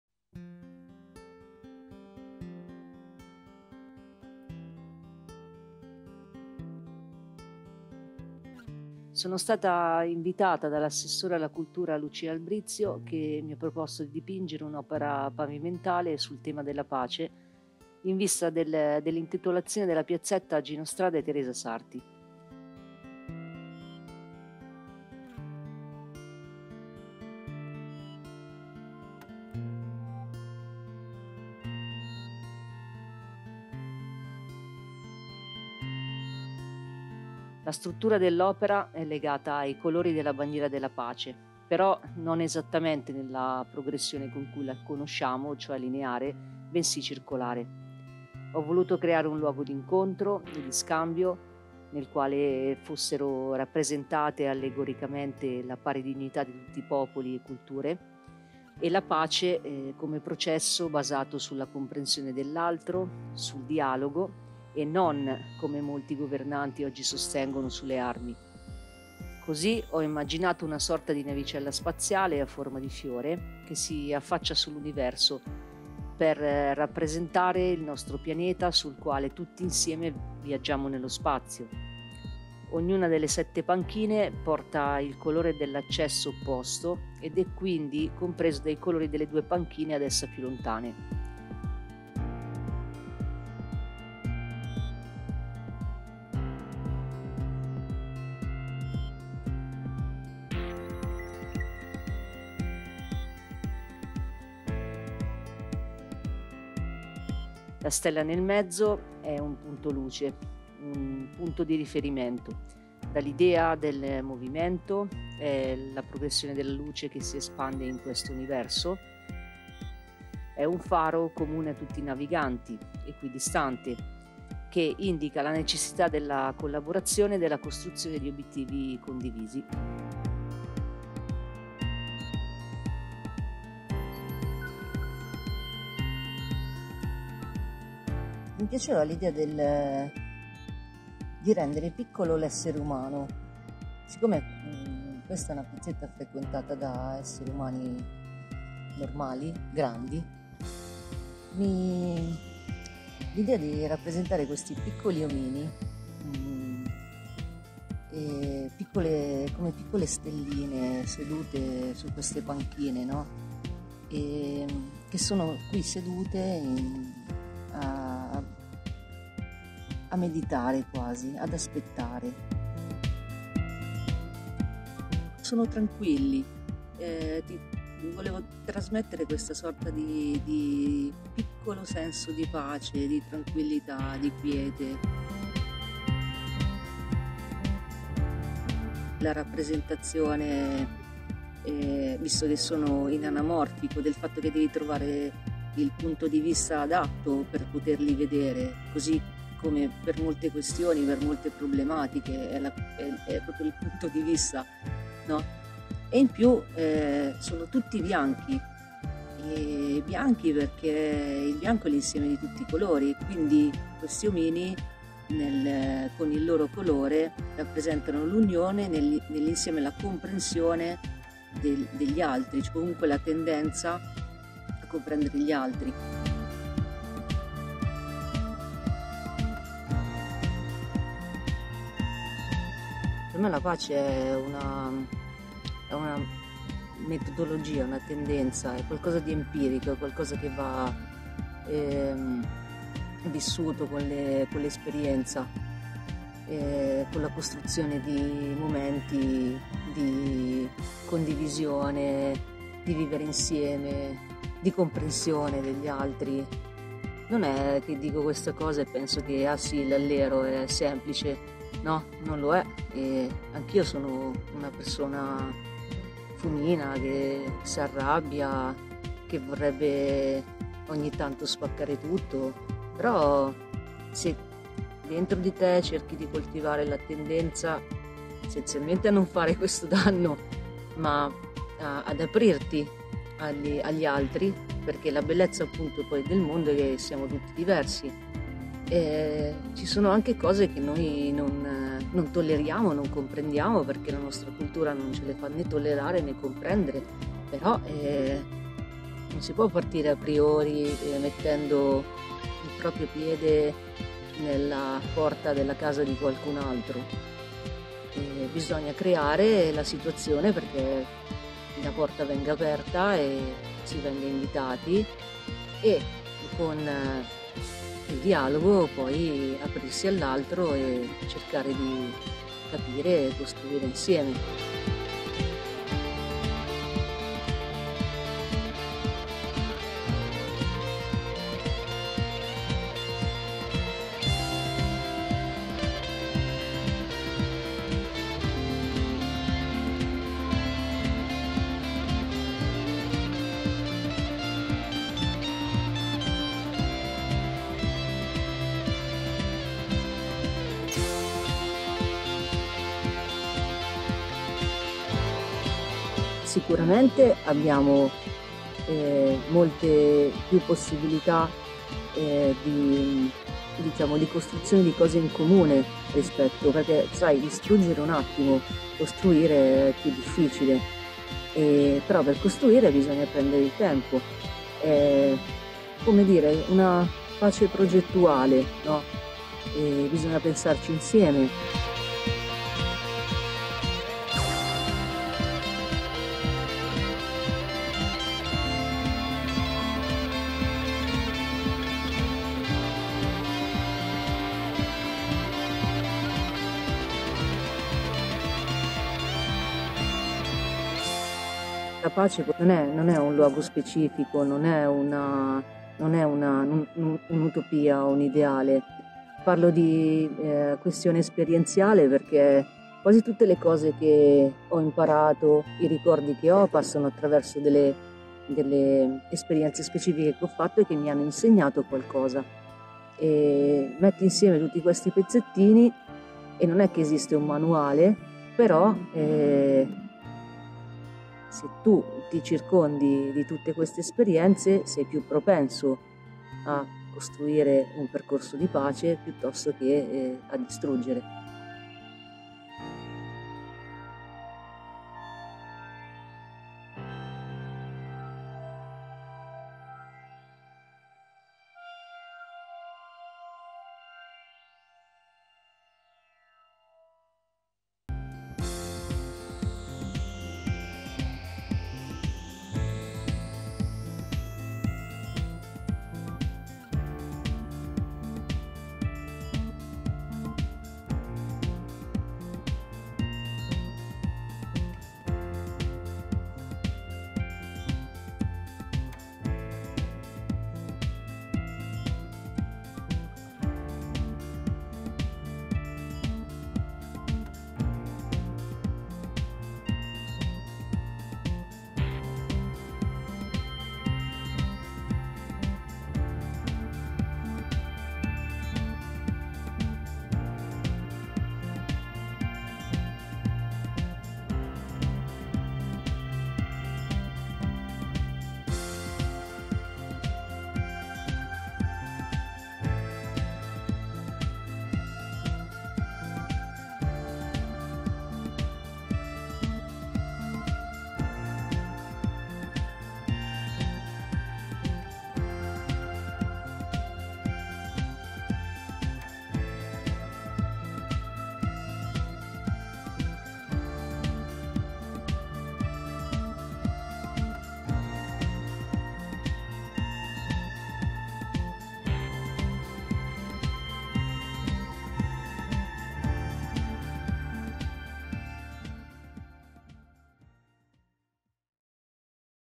si vis pacem para comprensionem
Presentazione dell'opera pavimentale sul tema della pace realizzata nel novembre 2022 a Bollate (MI).